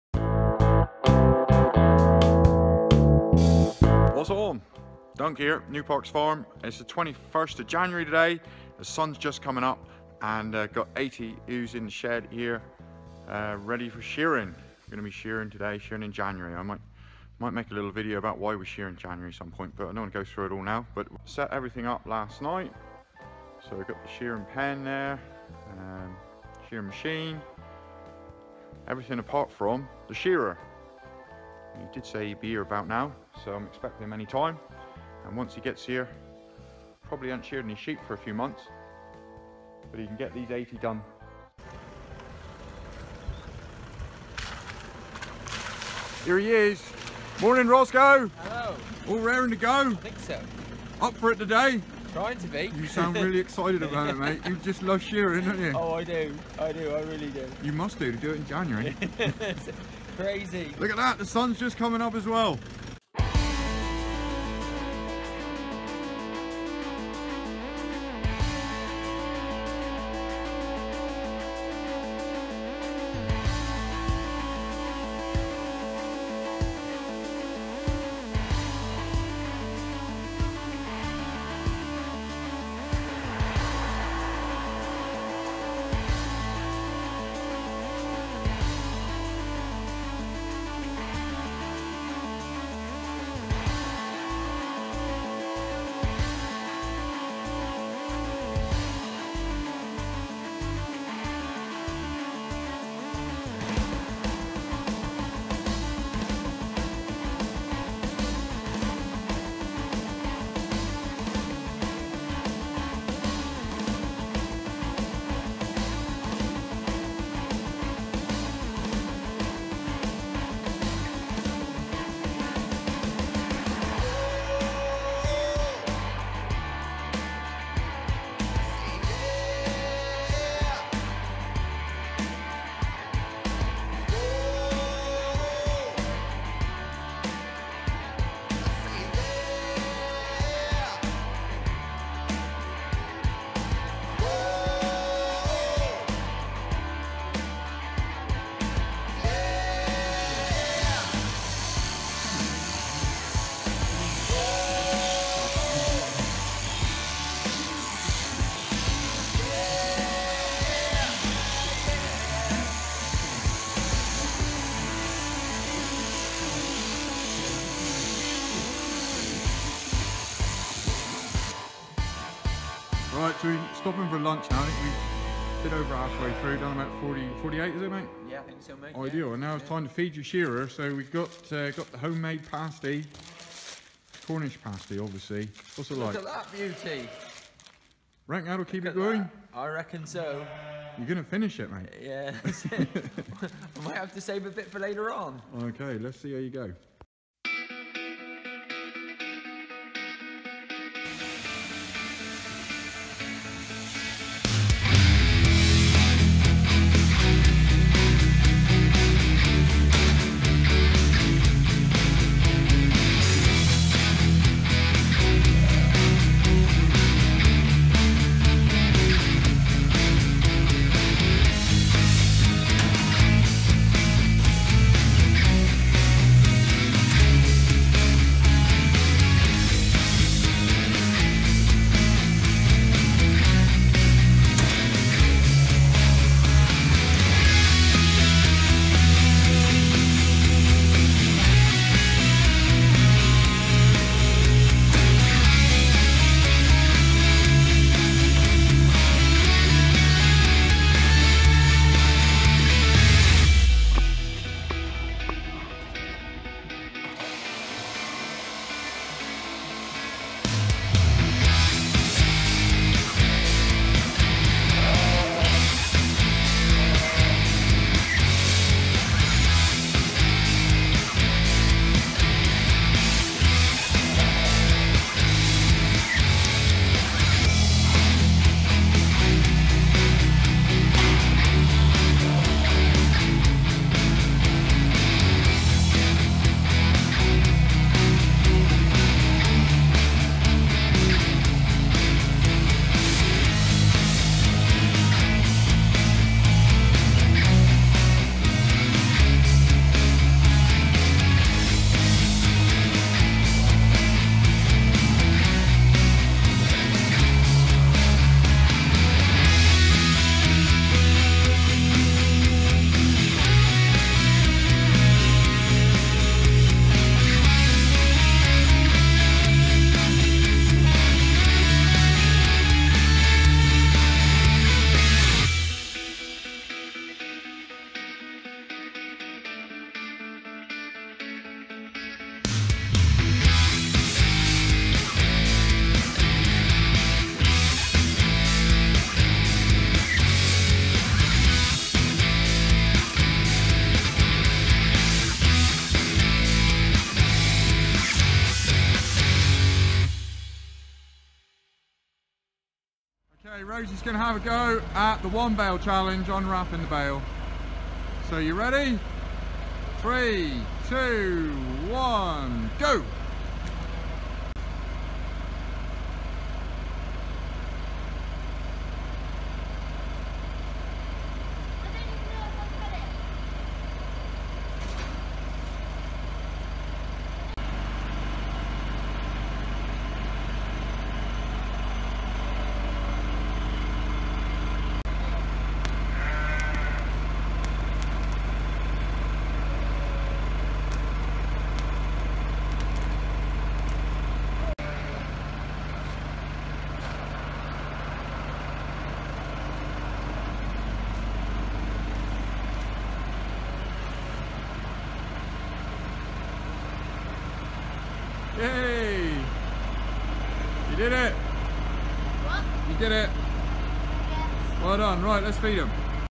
ROSCO eats huge Cornish Pasty and shears 80 sheep in January 2021, Cornwall farming life
Have you ever eaten a pasty this big and then done an afternoons hard work? Rosco has, check it out.

We shear our ewes in January/Feb for several years now and there are good reasons that I will explain in a future video but for now here is the action of our first day shearing in 2021. The remainder of our flock will be sheared in a few weeks, lambing in March/April.

Our daughter Rosie's 3rd favorite sheep job is shearing so she was keen to help and did a great job today.